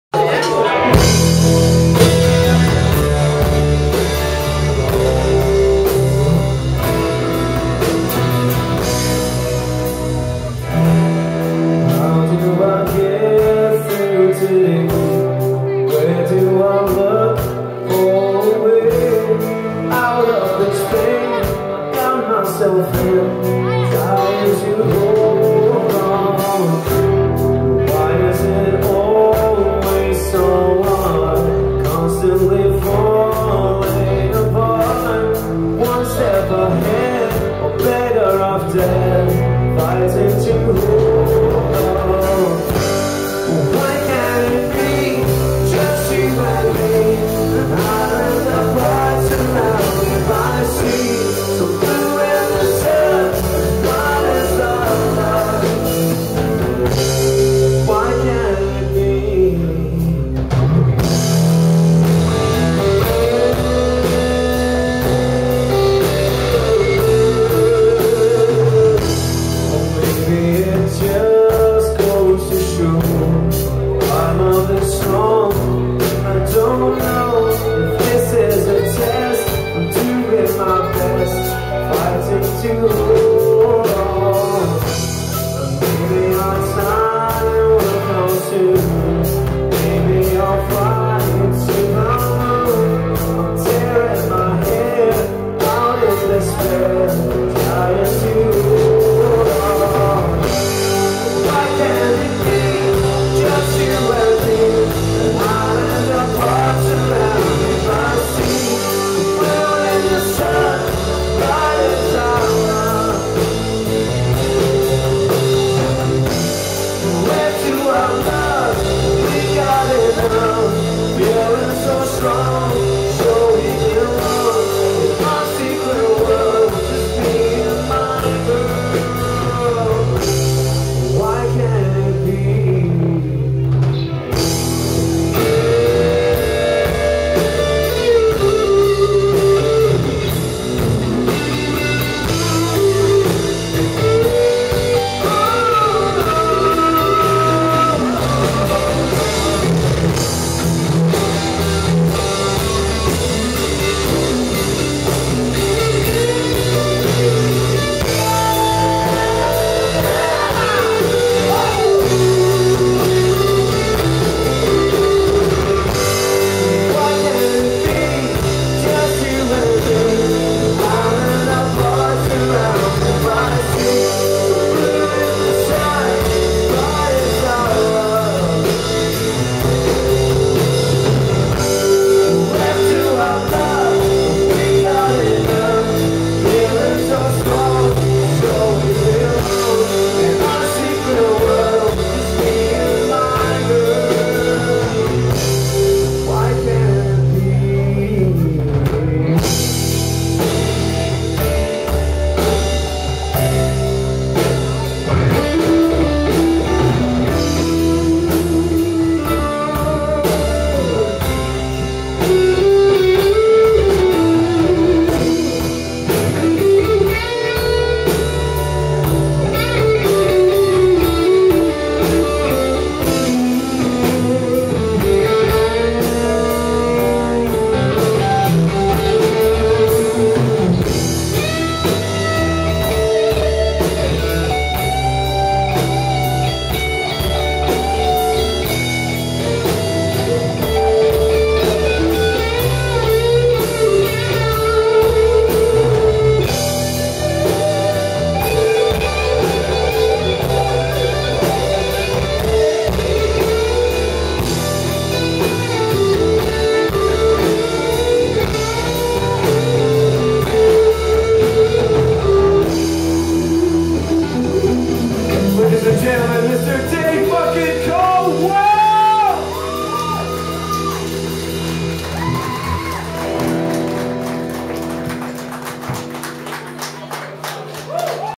Dave Bucket Colwell (4)
Dave Bucket Colwell live @ Bakers Kilmarnock